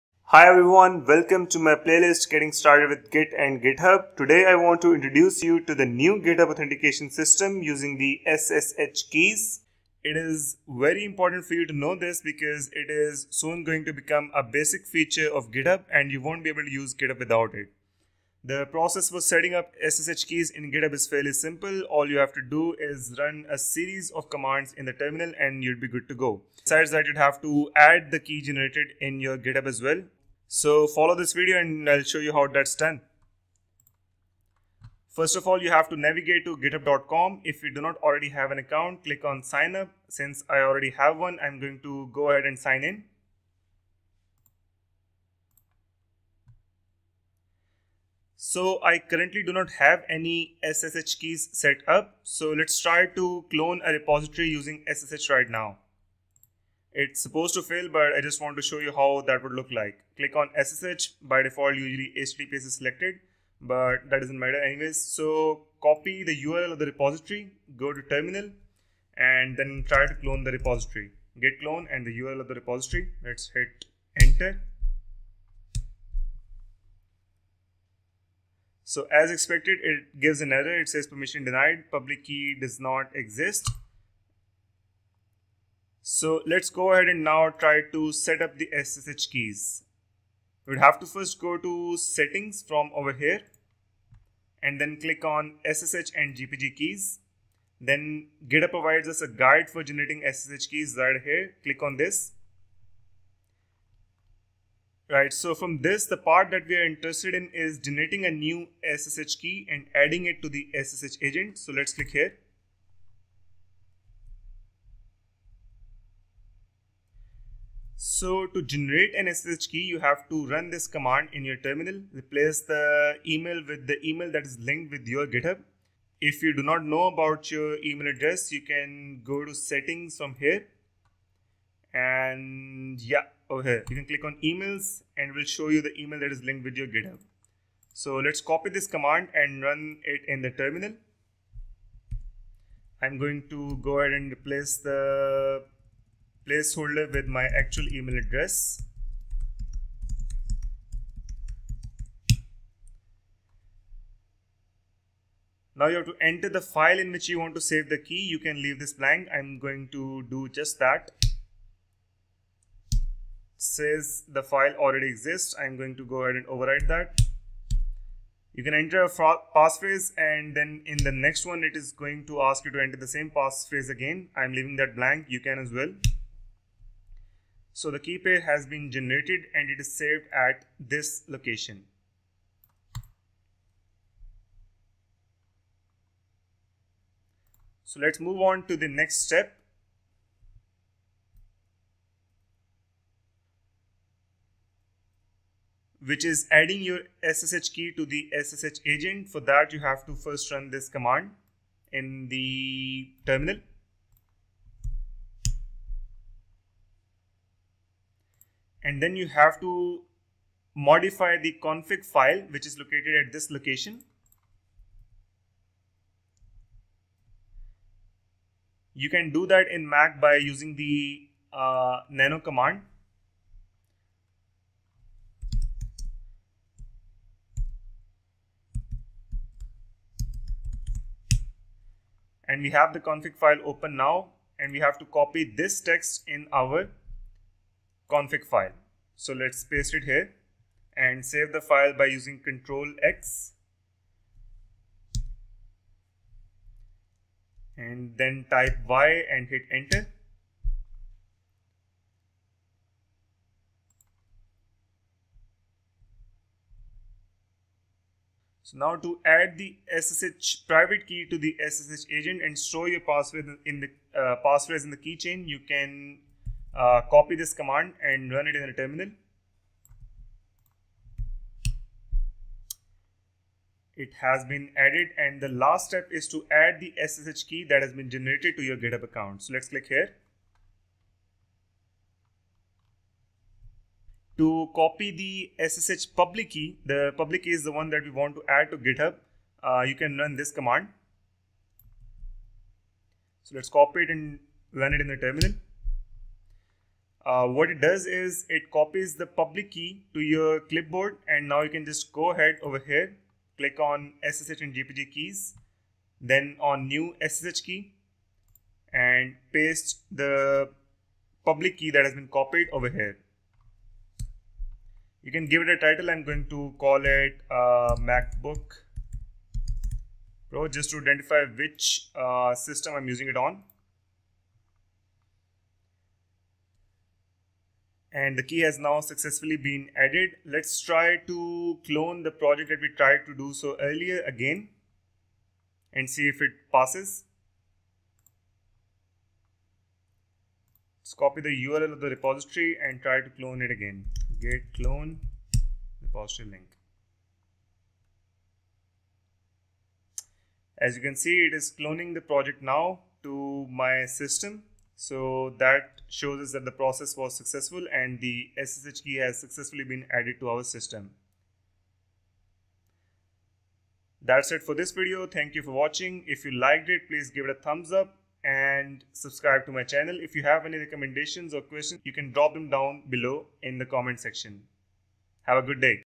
How to setup SSH for GitHub on Mac [2024]
Learn how to setup SSH for GitHub on mac by adding an SSH Key to your GitHub. Once done, you would not have to enter your username/password while pushing changes to your repositories.